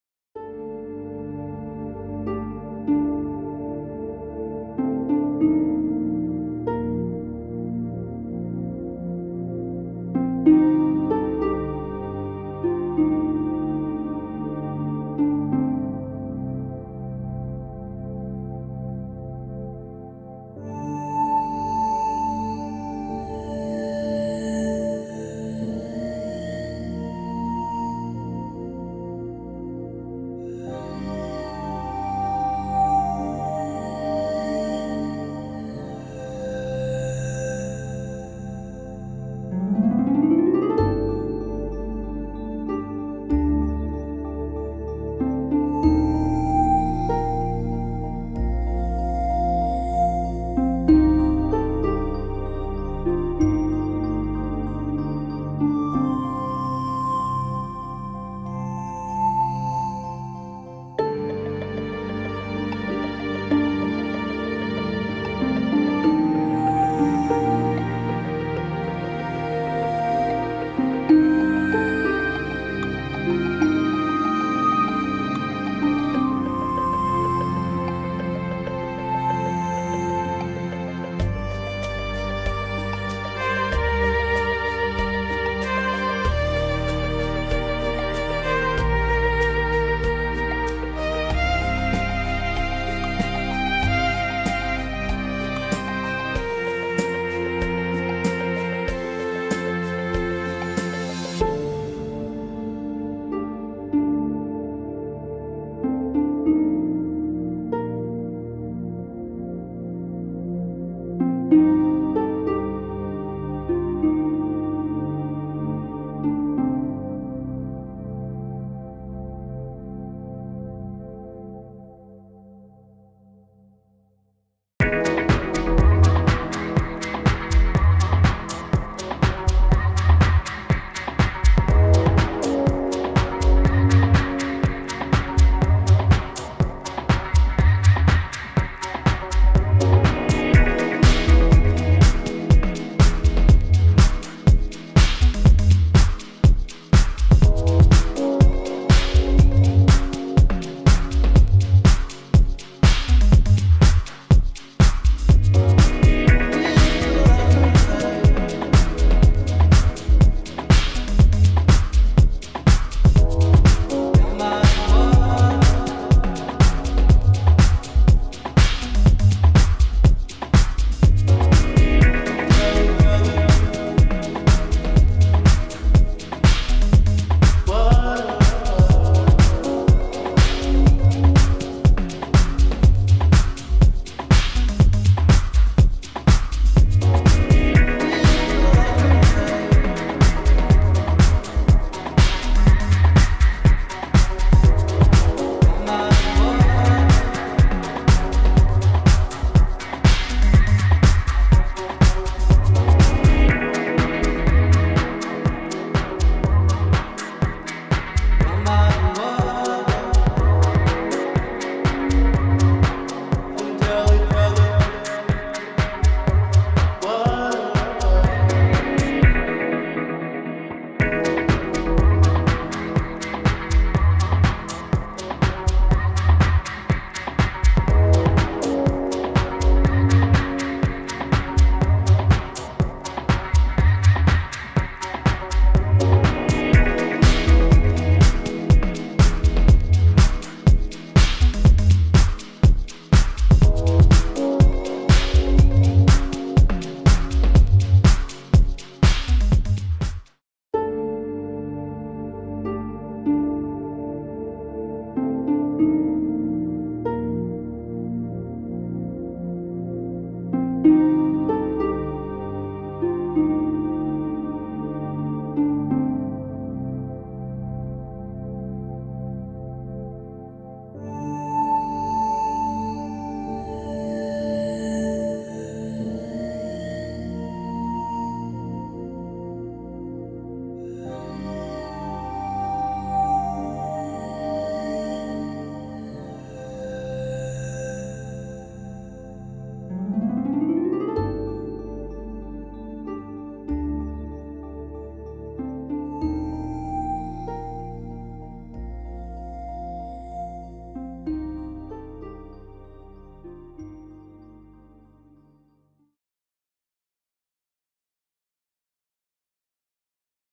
My First Drone Flight SD 480p
Filmed at Dobcross Primary School with DJI Mavic Mini 2